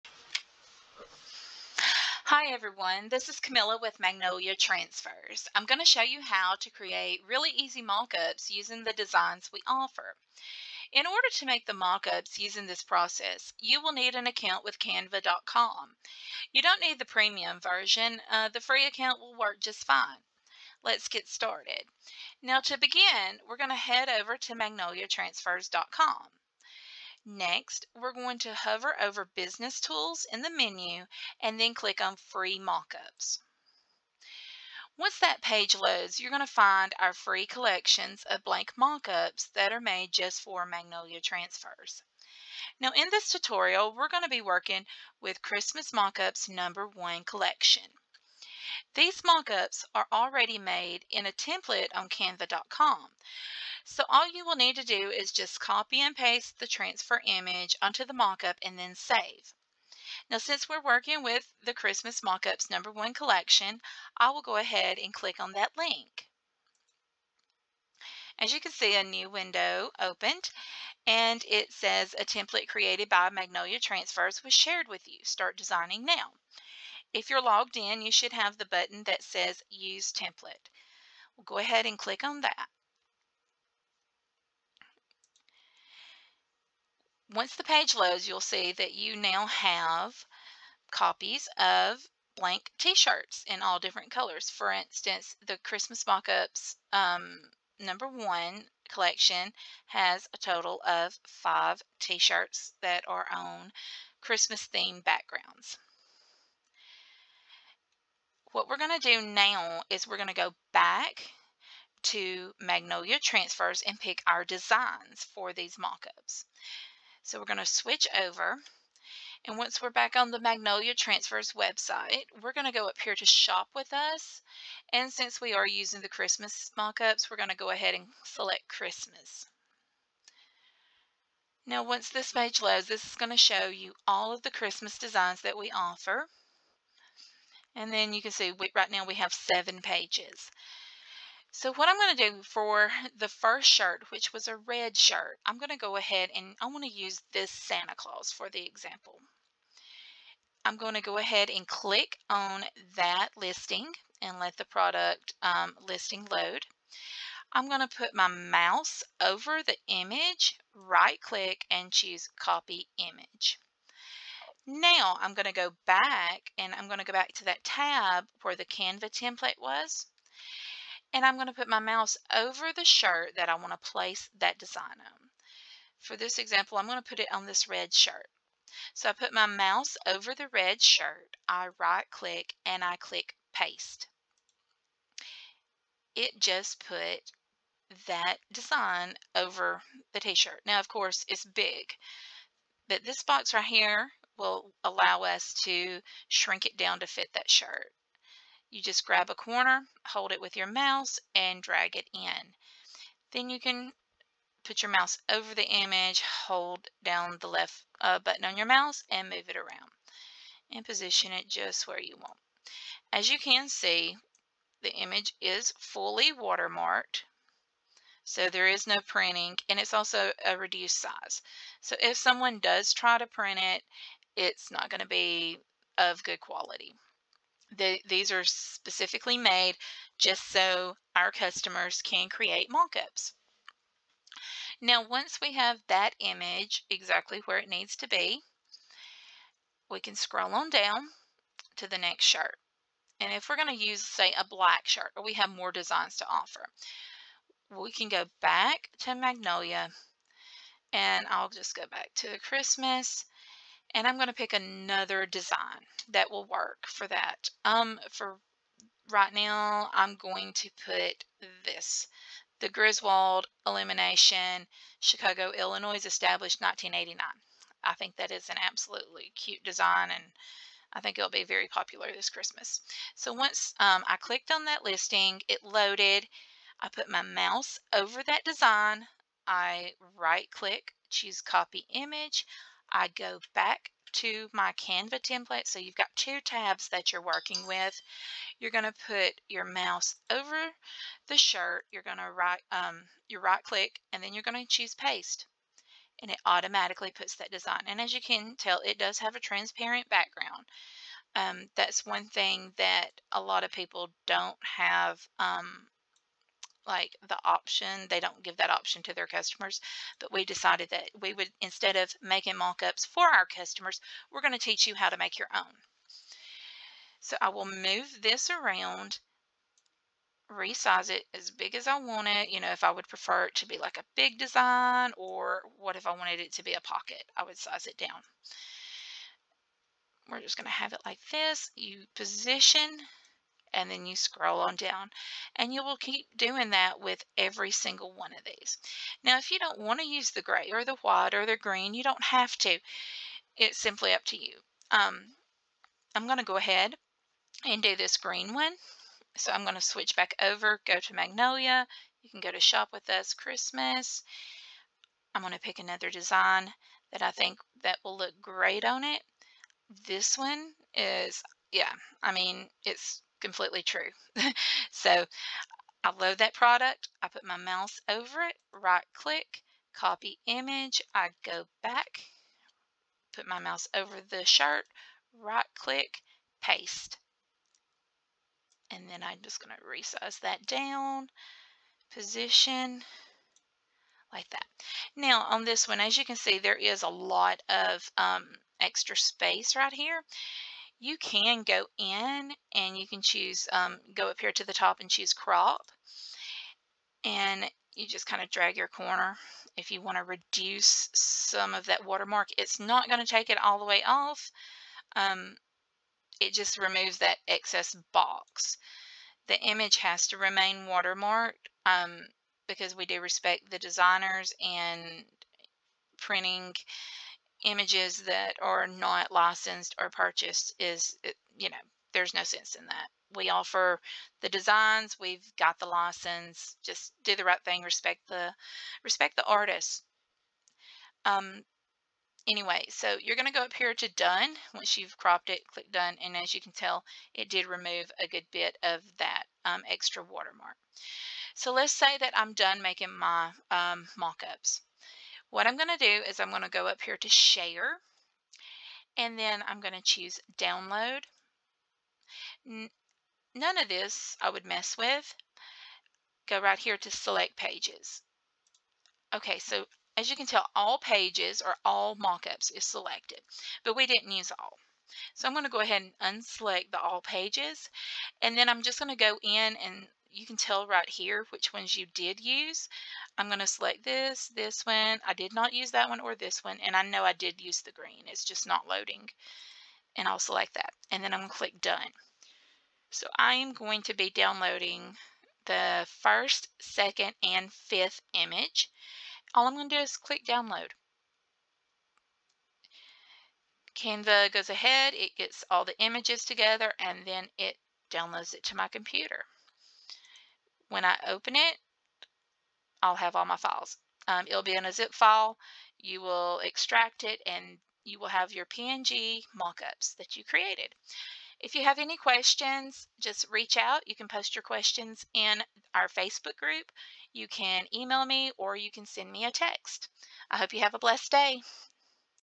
How To Make Shirt Mockups In Canva
Learn how to make t-shirt mockups using MagnoliaTransfers.com and Canva.